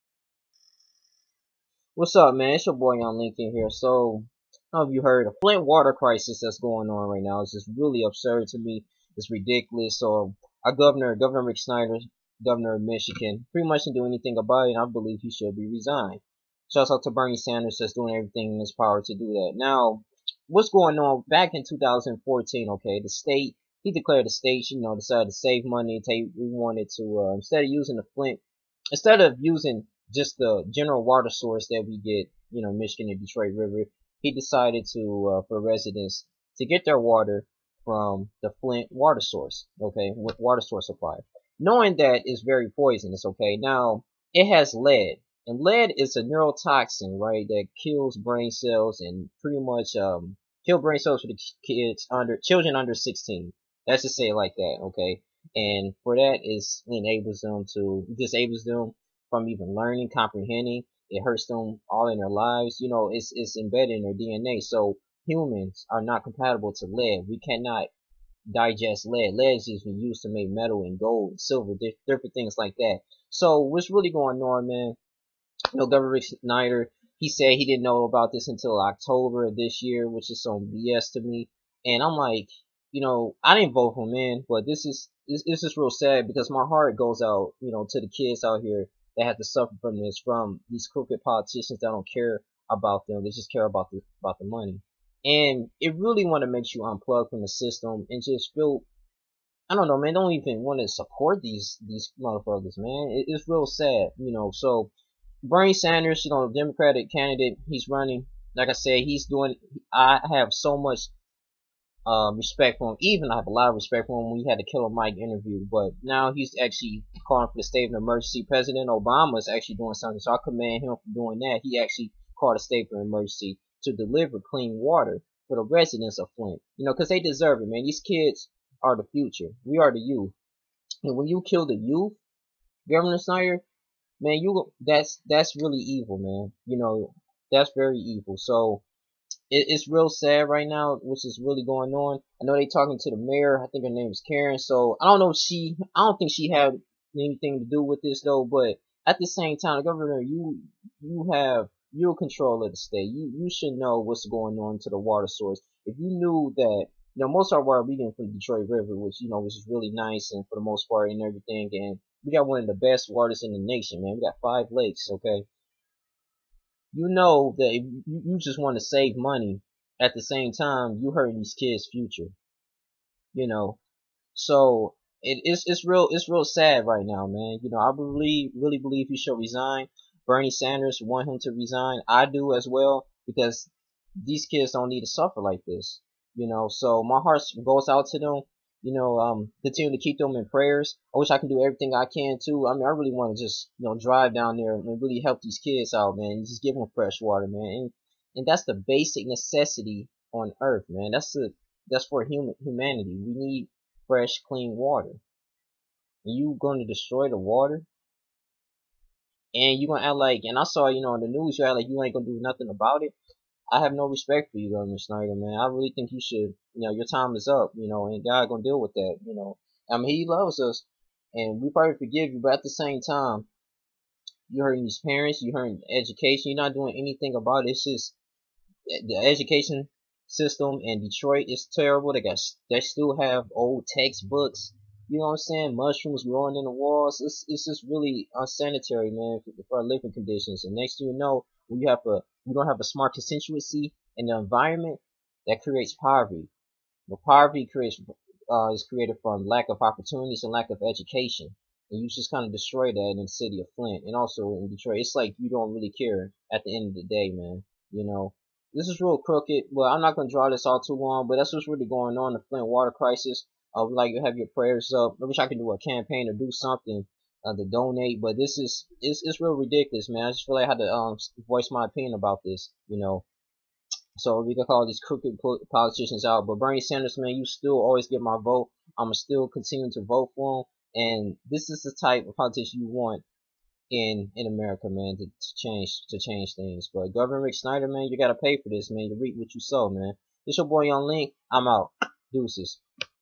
Flint Water Crisis
My heart is set to donate for the people in Flint.  Prayers will be with them all.

Governor Rick Snyder wanted the state to save money so decided to switch the water source to the flint river for the residents in that city.  Sadly the water has been contaminated with lead which is a neurotoxicn that causes brain damage to children under the age of 16.  This is an epidemic, thankfully president Obama called for an state of emergency for an solution.  Lets continue to pray for the residents.  God bless.

Facebook Rodney Jones Jr.